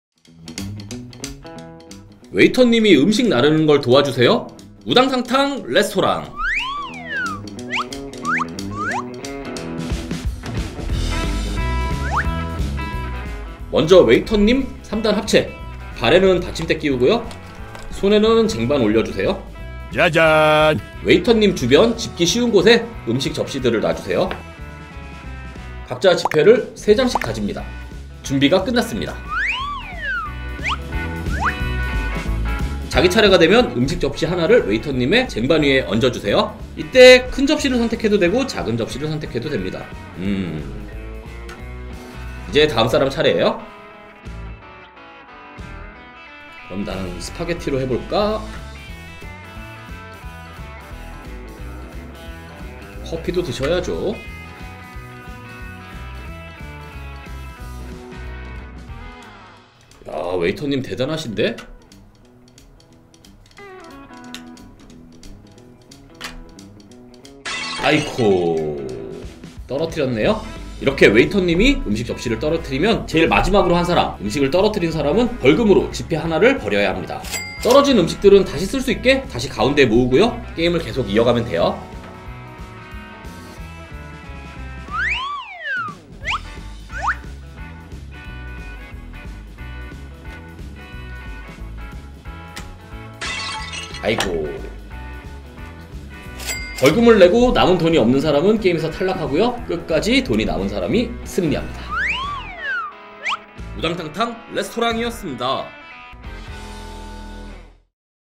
우당탕탕 레스토랑
레스토랑에 손님은 가득한데, 웨이터는 단 한 명!
아무래도 웨이터가 서빙하는 걸 도와줘야겠어요.
웨이터의 커다란 쟁반 위에 접시를 올려주자고요!

우당탕탕 레스토랑 규칙 영상입니다.